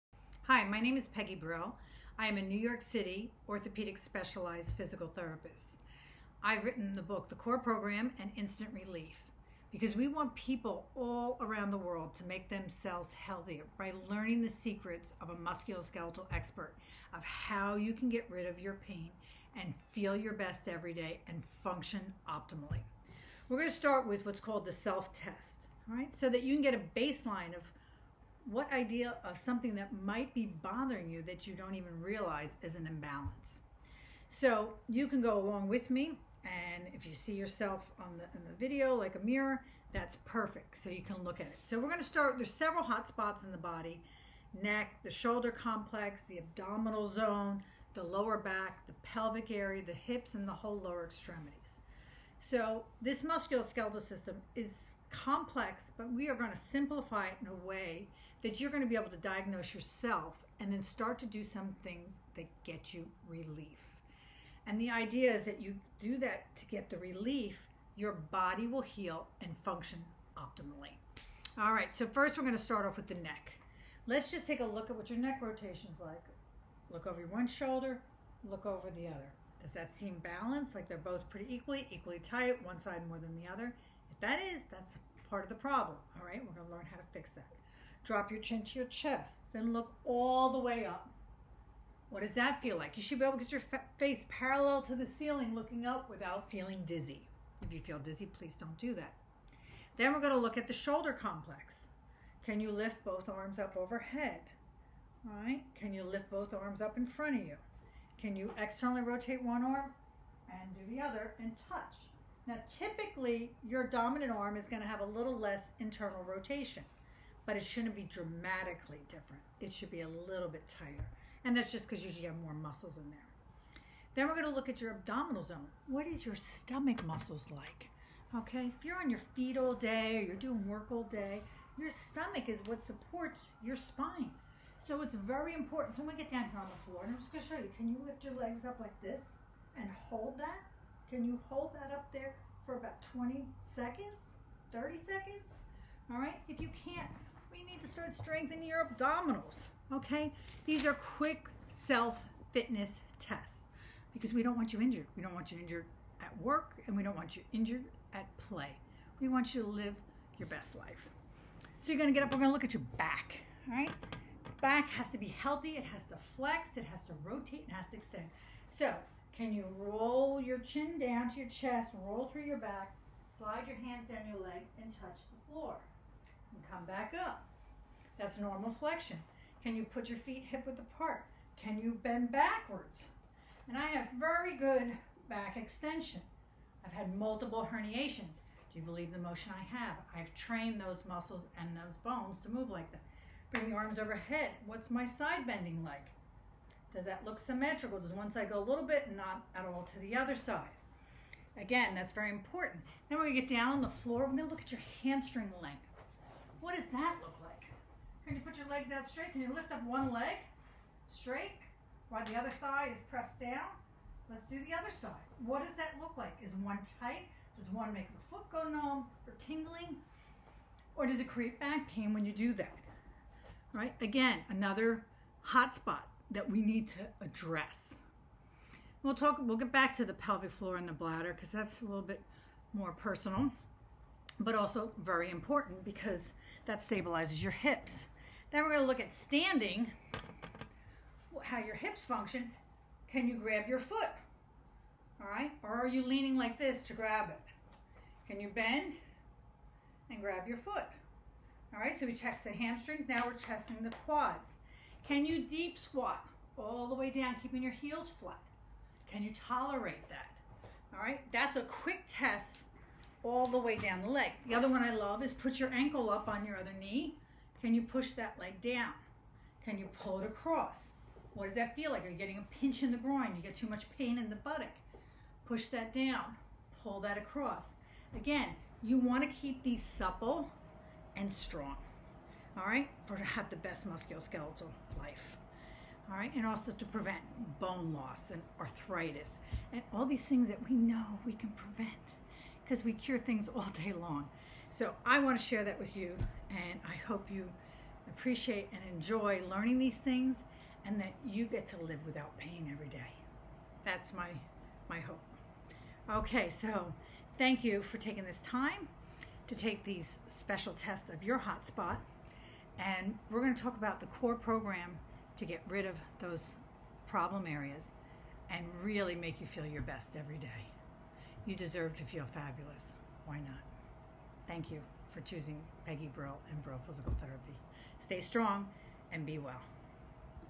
Musculoskeletal Self Test Screening!
Screen yourself with Peggy Brill, by doing these self tests to make
sure your in the best health for the New Year!